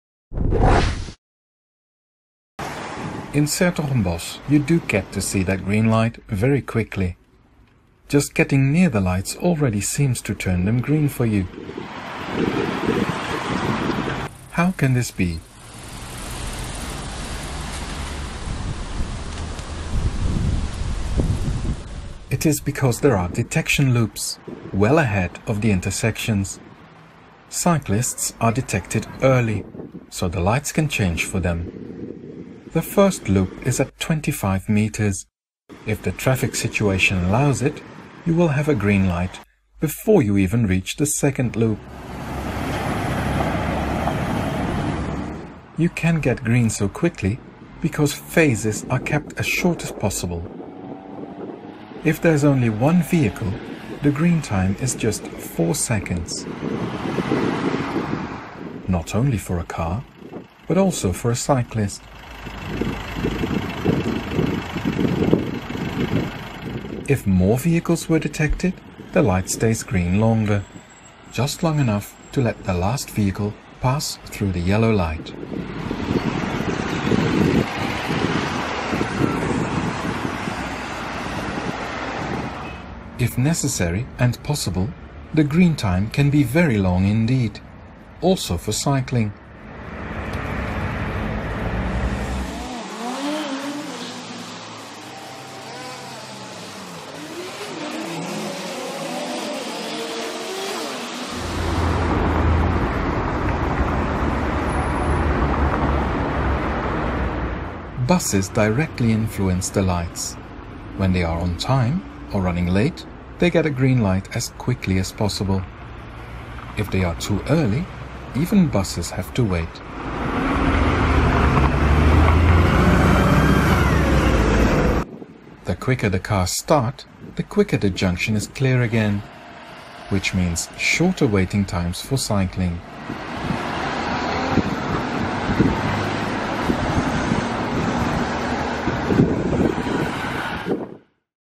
¿Un semáforo inteligente para no hacernos parar en vano?
En una ciudad de Holanda los semáforos cambian en función de cada usuario de la calle que llega a un cruce, independientemente de si es un coche, autobús, peatón o ciclista. Así funciona.